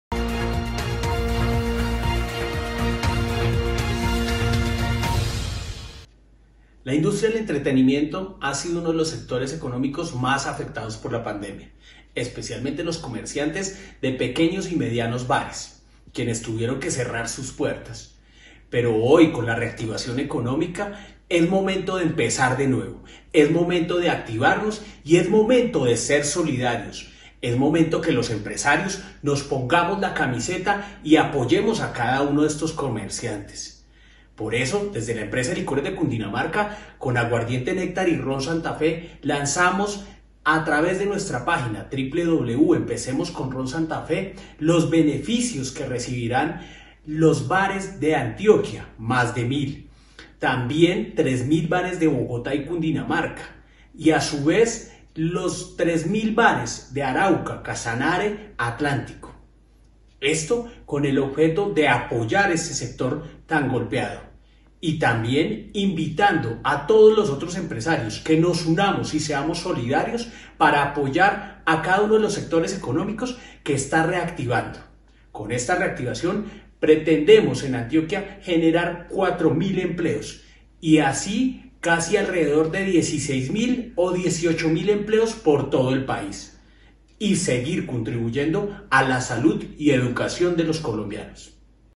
Ron Santafe y Aguardiente Nectar impulsan la reactivación de la industria nocturna y en Antioquia
Consciente de la enorme problemática que por cuenta de la pandemia enfrentan la industria nocturna y del entretenimiento en todo el país, y especialmente en Antioquia; Ron SantaFe, Aguardiente Nectar y el distribuidor Interamericana de Licores se han unido para emprender a partir de hoy una enorme campaña de apoyo a 1000 bares, gastrobares, restaurantes, discotecas y otros establecimientos a través de la entrega gratuita de producto para que estos lo comercialicen e inviertan estos recursos y las ganancias en su reactivación.